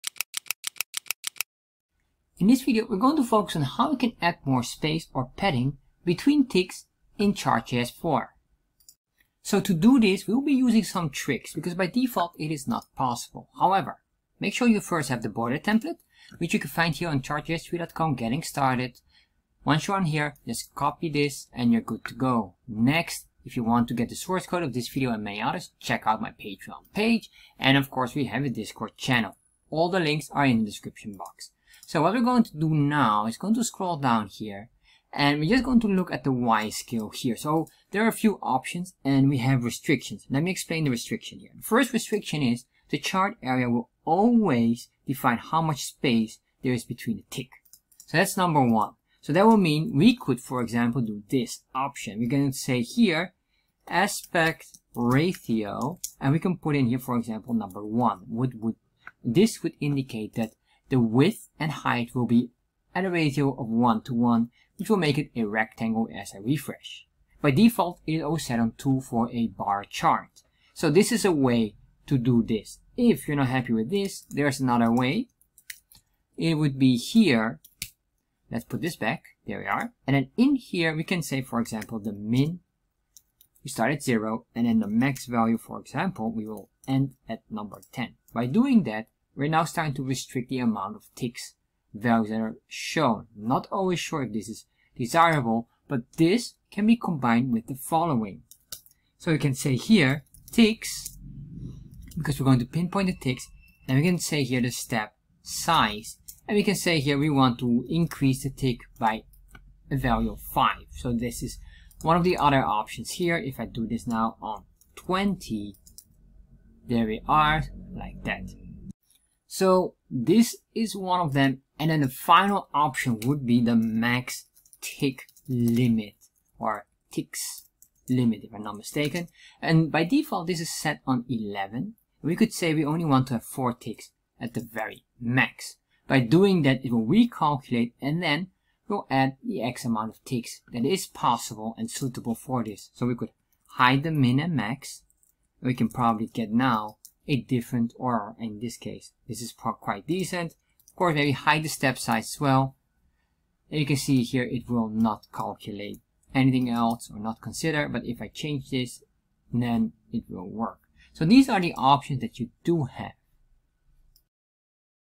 How to Add Padding Between Y Scale Ticks in Chart JS 4
In this video we will explore how to add padding between y scale ticks in chart js 4.

Chart js does not have a default option to do this. It does have some alternative options as the ticks are controlled by the chartArea and are precalculated.

▬ Materials/References  ▬▬▬▬▬▬▬▬▬▬

Chart JS Dashboard Series:
👍 Most Liked Video Series:

▬ About Us  ▬▬▬▬▬▬▬▬▬▬▬▬